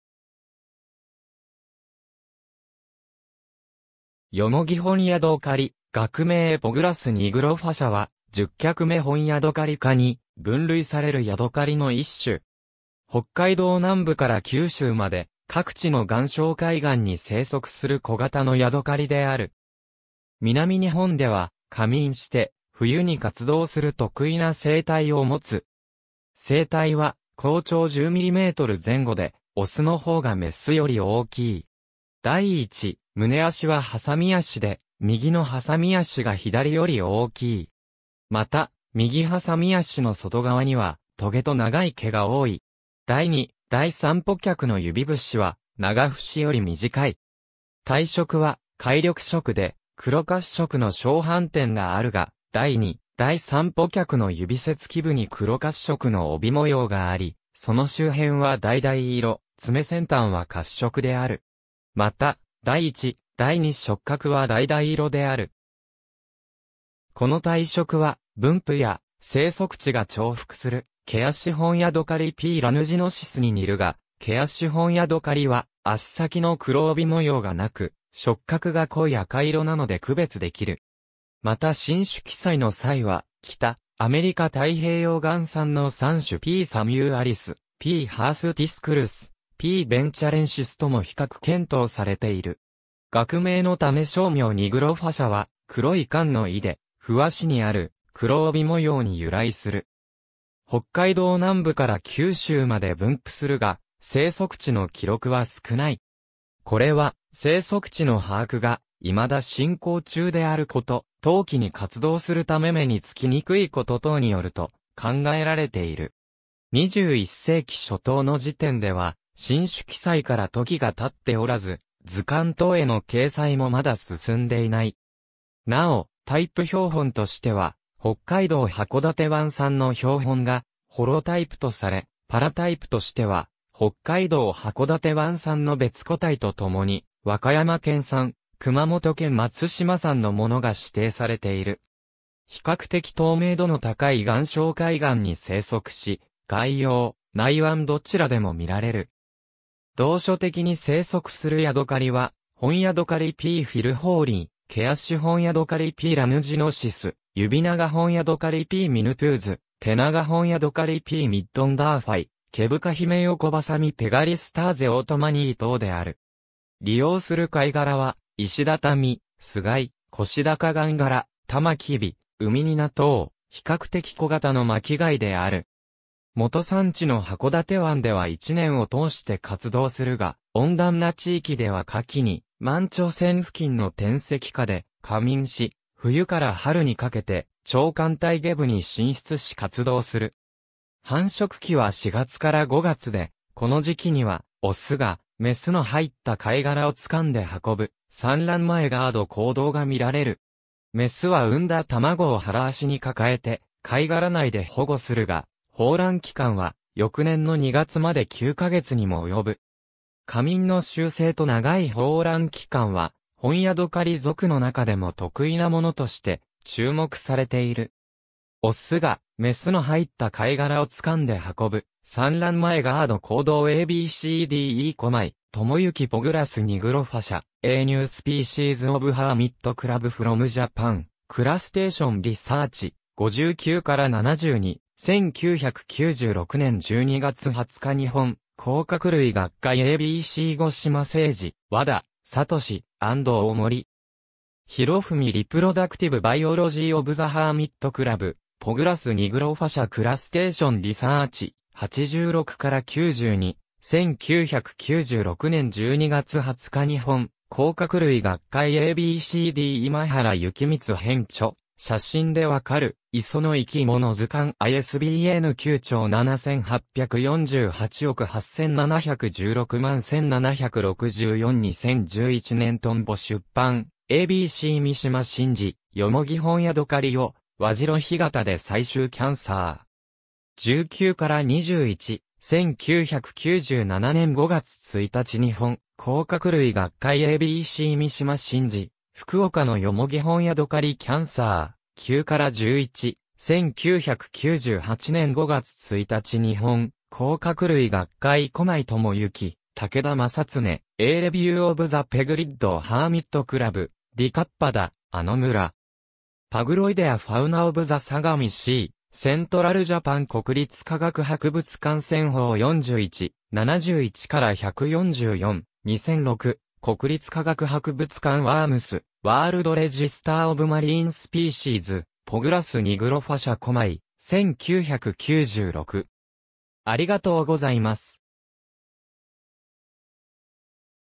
ヨモギホンヤドカリ
ヨモギホンヤドカリ（蓬本宿借、蓬本寄居虫）、学名 Pagurus nigrofascia は、十脚目ホンヤドカリ科に分類されるヤドカリの一種。
北海道南部から九州まで各地の岩礁海岸に生息する小型のヤドカリである。
南日本では「夏眠」して冬に活動する特異な生態をもつ。
成体は甲長10mm前後で、オスの方がメスより大きい。
第一胸脚は鉗脚で、右の鉗脚が左より大きい。
また右鉗脚の外側には棘と長い毛が多い。
第二・第三歩脚の指節は長節より短い（指節は脚先の爪がある関節、長節はその次の脛下半分にみえる関節）。
体色は灰緑色で、黒褐色の小斑点があるが、第二・第三歩脚の指節基部（爪になった関節の根元）に黒褐色の帯模様があり、その周辺は橙色、爪先端は褐色である。
また第一・第二触角は橙色である。
この体色は分布や生息地が重複するケアシホンヤドカリ P. lanuginosus に似るが、ケアシホンヤドカリは脚先の黒帯模様がなく、触角が濃い赤色なので区別できる。
また新種記載の際は、北アメリカ太平洋岸産の3種 P. samuelis、P. hirsutiusculus、P. venturensis とも比較検討されている。
学名の種小名"nigrofascia"は「黒い環」の意で、歩脚にある黒帯模様に由来する。
北海道南部から九州まで分布するが、生息地の記録は少ない。
これは生息地の把握が未だ進行中であること、冬季に活動するため目につきにくいこと等によると考えられている。
21世紀初頭の時点では新種記載から時が経っておらず、図鑑等への掲載もまだ進んでいない。
なおタイプ標本としては、北海道函館湾産の標本がホロタイプとされ、パラタイプとしては北海道函館湾産の別個体とともに和歌山県産・熊本県松島産のものが指定されている。
比較的透明度の高い岩礁海岸に生息し、外洋・内湾どちらでも見られる。
同所的に生息するヤドカリはホンヤドカリ P. filholi（外洋性）、ケアシホンヤドカリ P. lanuginosus（外洋・内湾性）、ユビナガホンヤドカリ P. minutus（内湾性）、テナガホンヤドカリ P. middendorffii（寒海生）、ケブカヒメヨコバサミ Paguristes ortmanni（外洋・内湾性） 等である。
利用する貝殻はイシダタミ、スガイ、コシダカガンガラ、タマキビ、ウミニナ等、比較的小型の巻貝である。
基産地の函館湾では1年を通して活動するが、温暖な地域では夏季に満潮線付近の転石下で「夏眠」し、冬から春にかけて潮間帯下部に進出し活動する。
繁殖期は4月-5月で、この時期にはオスがメスの入った貝殻を掴んで運ぶ「産卵前ガード」行動が見られる。
メスは産んだ卵を腹脚に抱えて貝殻内で保護するが、抱卵期間は翌年の2月まで9ヶ月にも及ぶ。
夏眠の習性と長い抱卵期間は、ホンヤドカリ属の中でも特異なものとして注目されている。
オス（大・右）がメス（小・左）の入った貝殻を掴んで運ぶ「産卵前ガード」行動（4月） ^ a b c d e Komai Tomoyuki"Pagurus nigrofascia, a new species of hermit crab (Decapoda: Anomura: Paguridae) from Japan"(日本産ホンヤドカリ属の一新種) Crustacean research (25), 59-72, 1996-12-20 日本甲殻類学会 ^ a b c Goshima Seiji, Wada Satoshi, and Ohmori
Hirofumi "Reproductive biology of the hermit crab Pagurus nigrofascia (Anomura: Paguridae)"(ヨモギホンヤドカリの繁殖生態) Crustacean research (25), 86-92, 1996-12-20 日本甲殻類学会 ^ a b c d 今原幸光編著『写真でわかる磯の生き物図鑑』（解説 : 大谷道夫）ISBN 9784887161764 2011年 トンボ出版 ^ a b c 三島伸治 『ヨモギホンヤドカリを和白干潟で採集』 Cancer(6),
19-21, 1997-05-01 日本甲殻類学会 ^ a b c 三島伸治 『福岡のヨモギホンヤドカリ』 Cancer(7), 9-11, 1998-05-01 日本甲殻類学会 ^ Komai Tomoyuki, Takeda Masatsune...